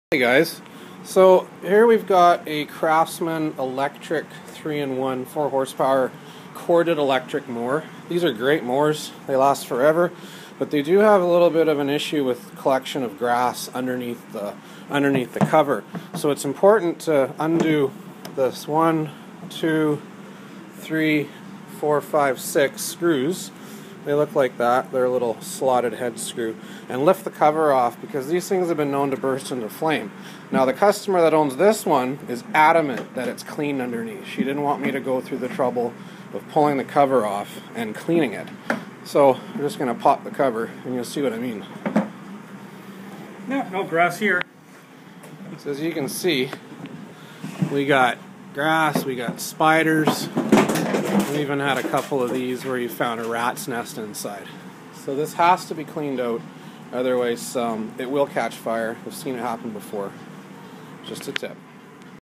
Black and Decker Electric Lawnmower Service
Why it is important to open up your Black and Decker electric lawnmower hood each season and clean it out. I've seen some of these machines catch fire and burn down garden sheds.